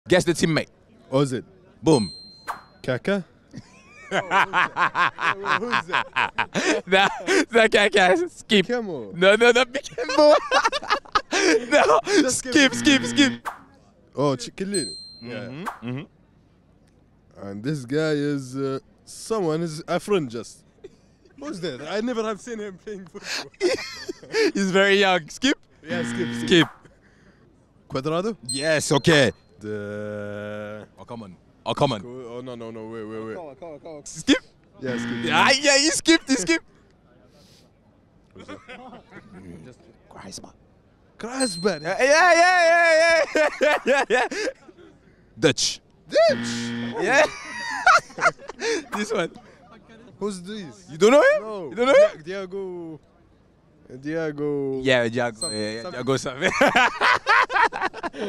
IT'S A DISASTER 😭 RONALDO'S TEAM-MATES CHALLENGE
Premier League football fan has a nightmare performance on the name the team-mates soccer challenge, trying to name nine players who played with Real Madrid, Manchester United, Juventus and Al-Nassr legend Cristiano Ronaldo!